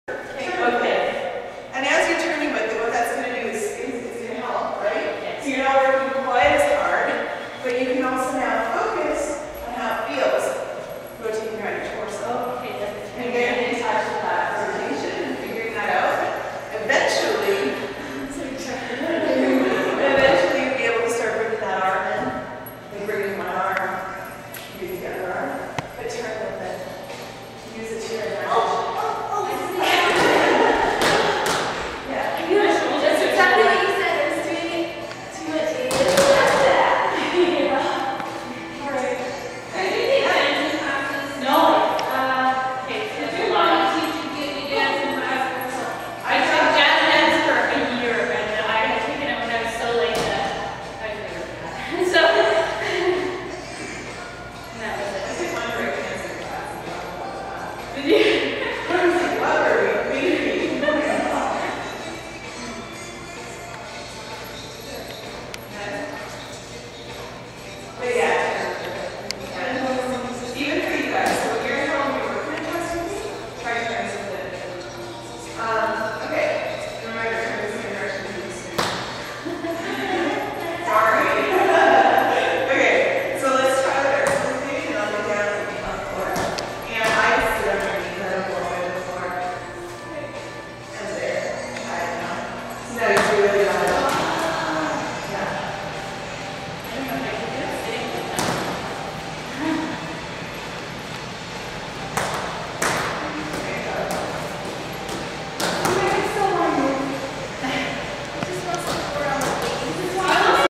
Flowscience Hoopdancing class at Britannia-Youngstown Community Centre
Carla Snow teaches hoopdance to her class of beginners at the Britannia-Youngstown Community Centre. The class meets every Monday, visitors are welcomed.